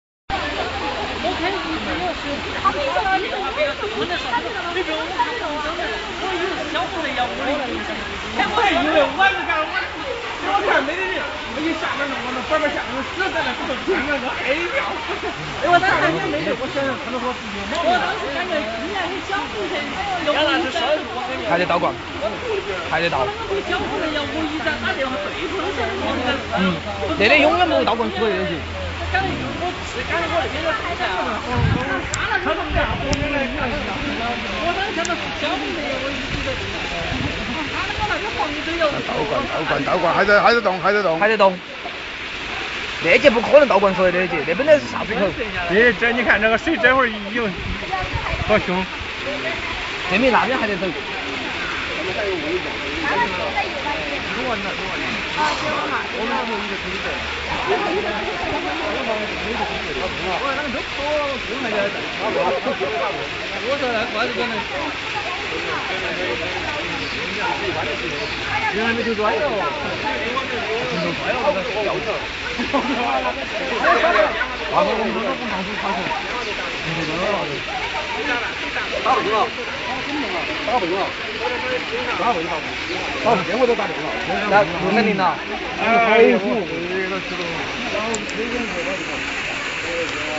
Escape from Sichuan, China earthquake (LIVE footage filmed while running with handycam)
the footage is shot by a tourist in Chongqing, showing that the water flow of the brook began to reverse when the earthquake happened.

I got caught in an aftershock during the Sichuan earthquake, this is my escpe. At the time I was reporting for CCTV9 at the epicentre of the massive 2008 ear.

this is a footage shot by a surveillance camera somewhere in the city of Chengdu, Sichuan Province in China during the earthquake happened on May 12, 2008.

Live footage taken DURING the SiChuan earthquake. Two university students took this video with a camera cell phone during the 7.8 earthquake in Sichuan China.